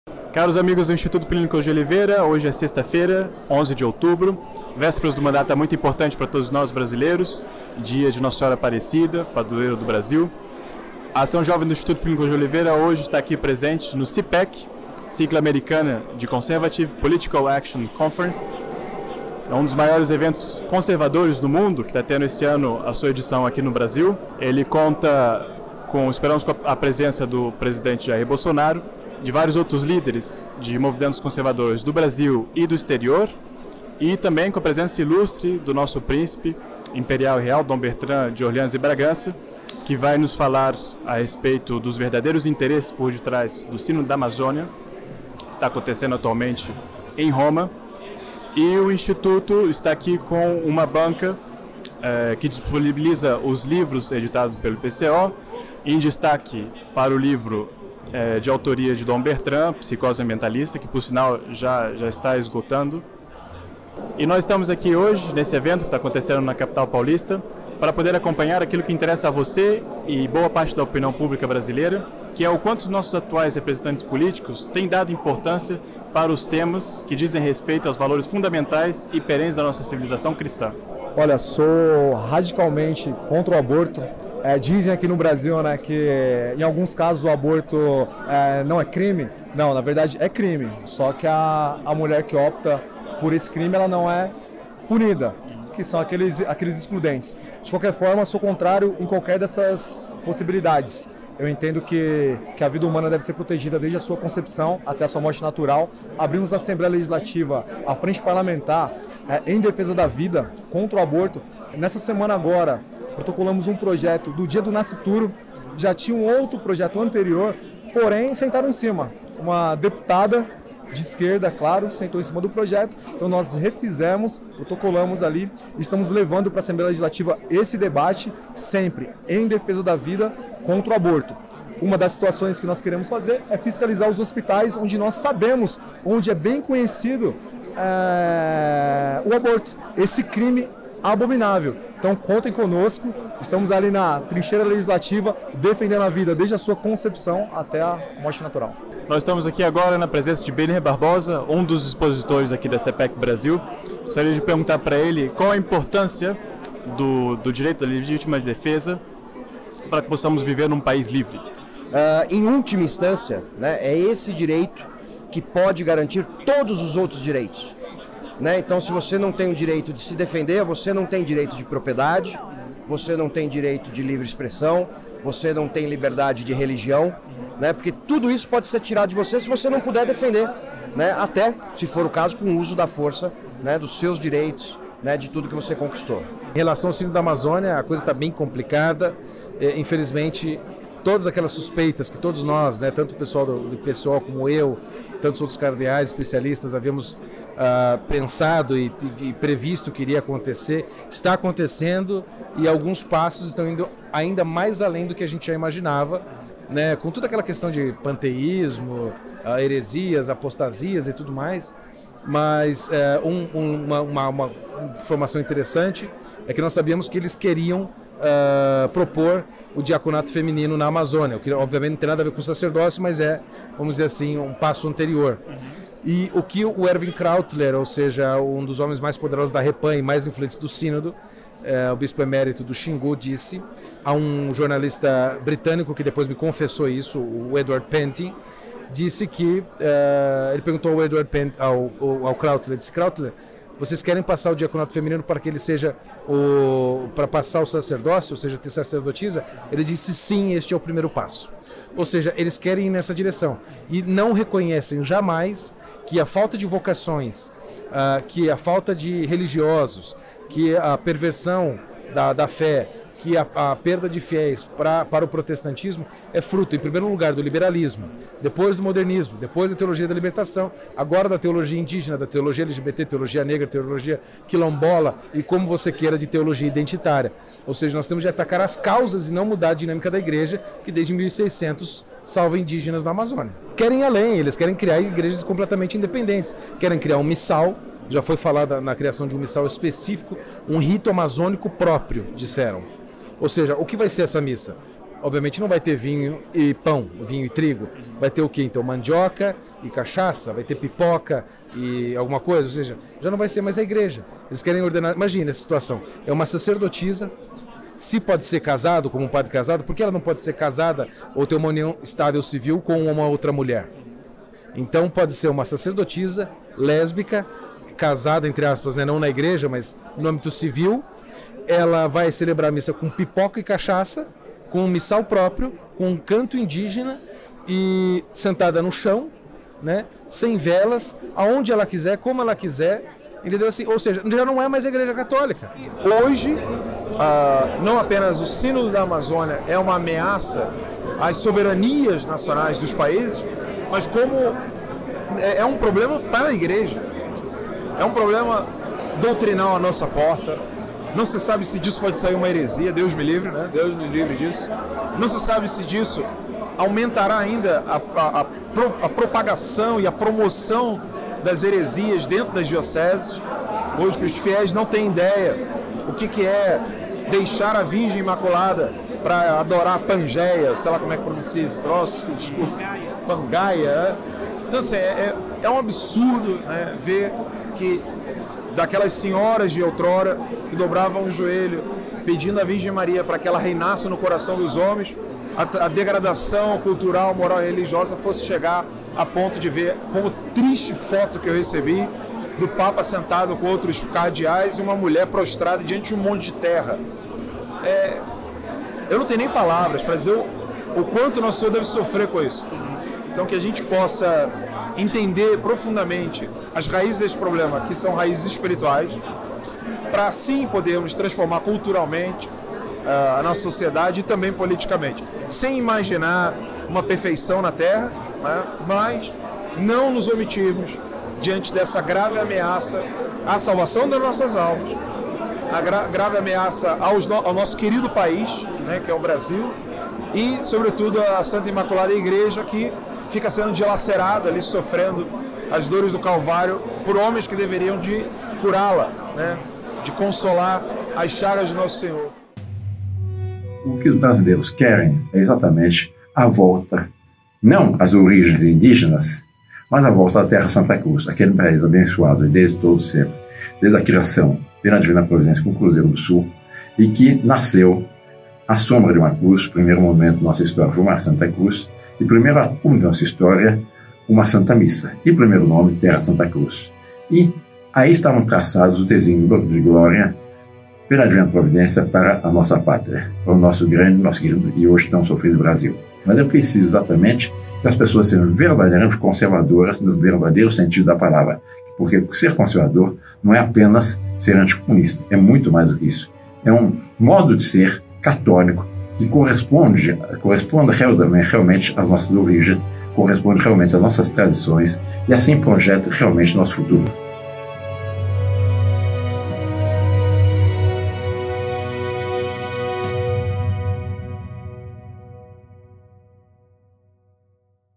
Conservadores: Uni-vos em defesa do Brasil e da Civilização - O Futuro é a Terra de Santa Cruz
Dom Bertrand, Bernardo P Küster, Dep. Gil Diniz, Bene Barbosa, Allan dos Santos dão entrevista ao Canal do IPCO durante o CPAC, em S. Paulo.

A ação Jovem do Instituto Plinio Correa de Oliveira esteve presente no CPAC Brasil (Conservative Political Action Conference) que teve sua primeira edição na capital paulista nos dias 11 e 12 de outubro.

Ajude a manter as atividades do IPCO. Veja os benefícios de ser parte dessa Cruzada pelo Brasil e pela Civilização Cristã